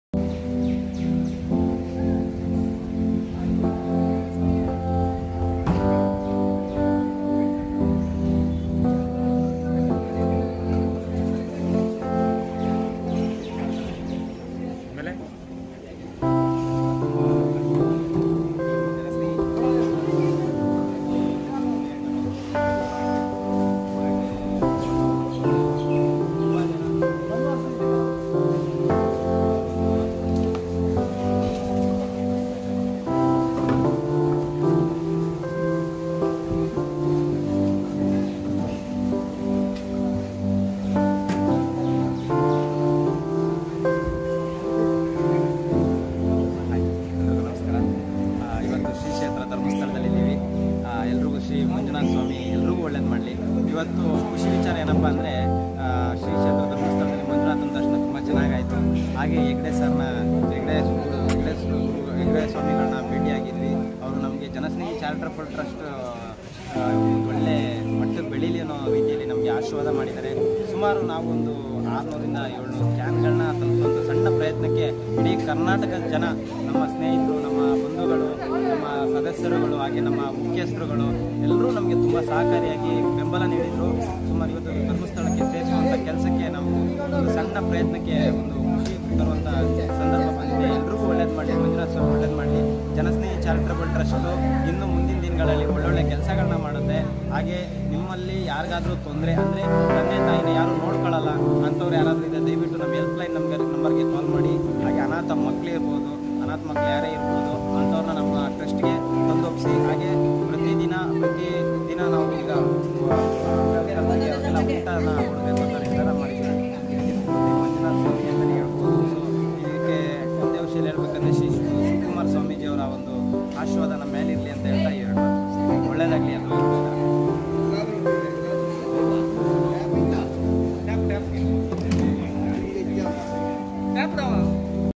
A Small Contribute to Sri Kshetra Dharmasthala
As there was a scarcity for water at Sri Kshetra Dharmasthala we contributed a 20K Litres of water .